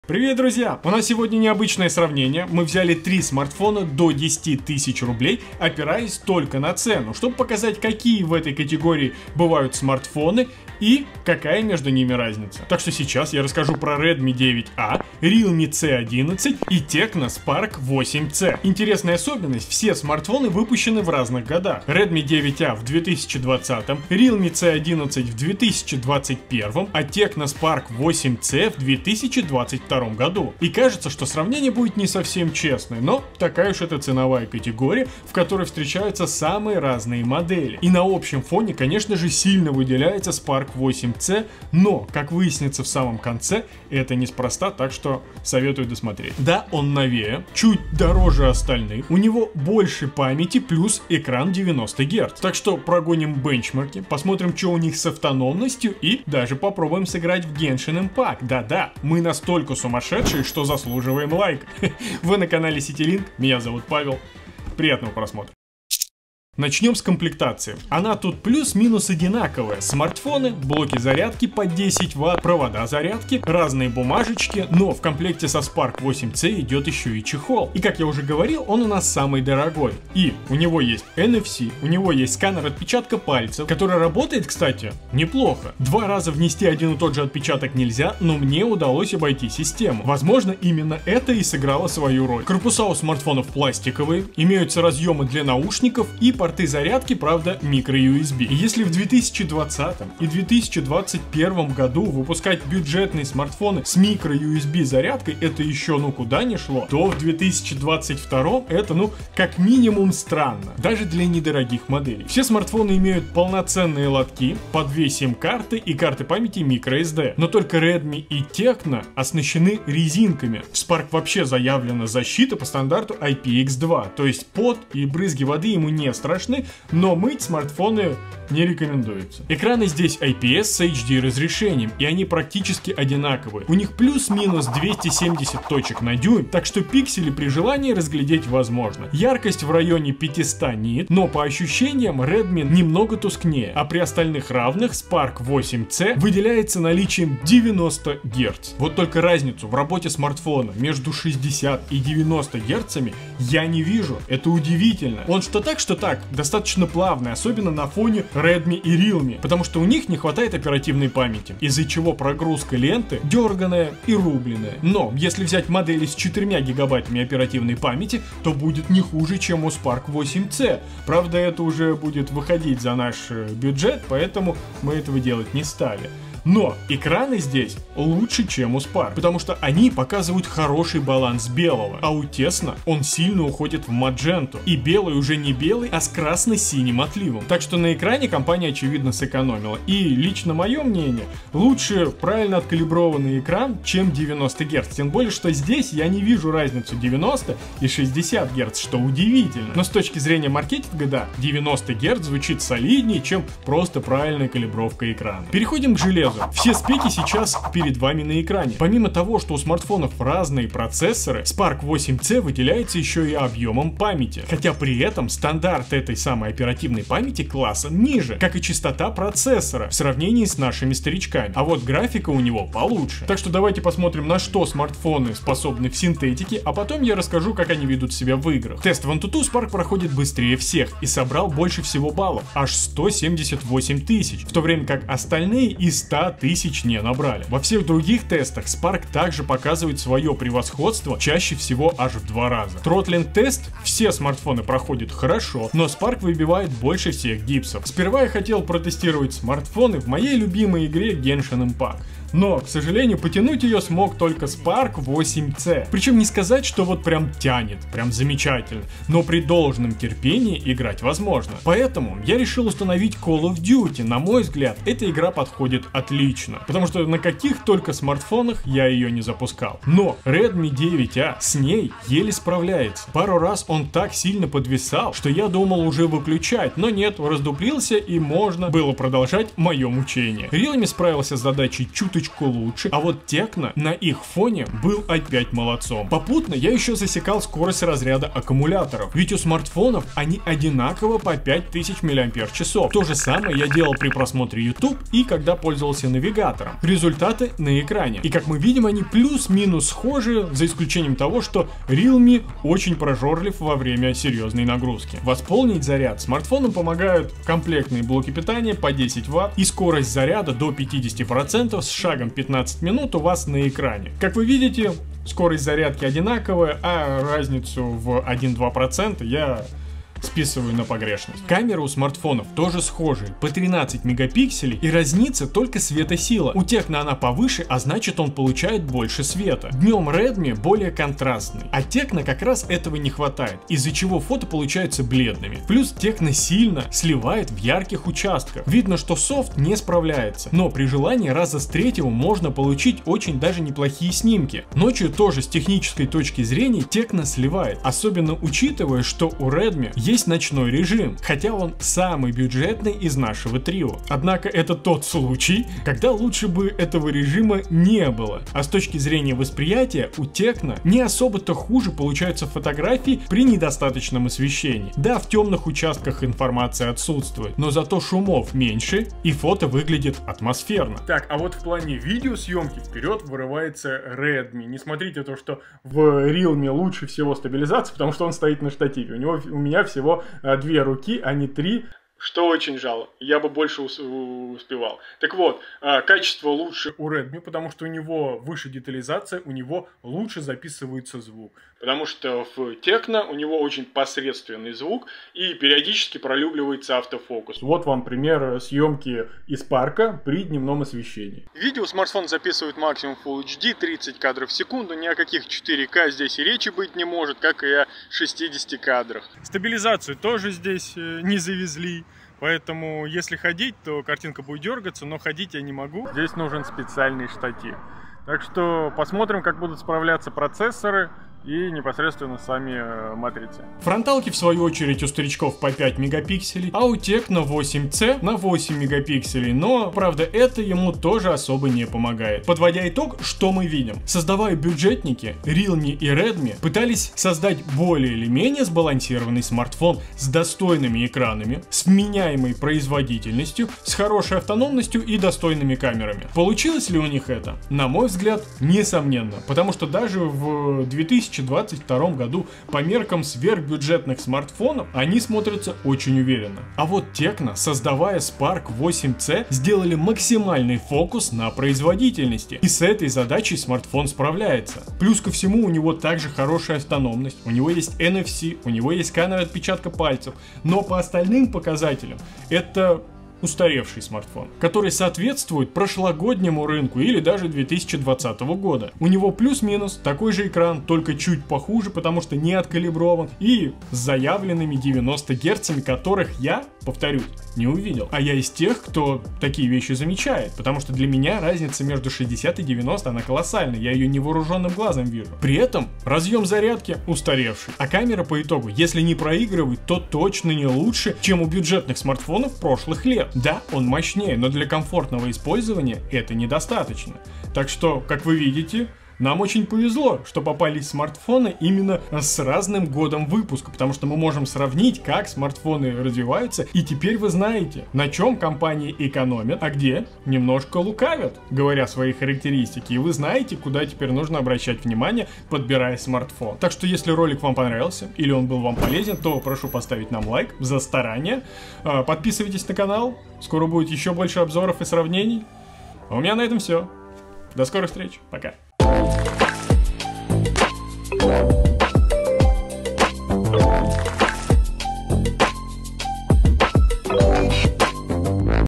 Сравниваем смартфоны ДО 10000 рублей!
00:00 - Приветствие
01:06 - Комплектация и характеристики
02:08 - Дисплей
03:26 - Железо
04:45 - Аккумулятор
05:20 - Камера
07:12 - Итоги

В этом видео мы сравним смартфоны, до 10000 руб., опираясь только на цену, чтобы показать какие смартфоны существуют в этой ценовой категории, и какая между ними разница. Сегодня мы сравним 3 смартфона по всем характеристикам: Redmi 9A, Realme C11 и TECNO SPARK 8C

Товар: [#1693386, #1687559, #1687562, #1402120]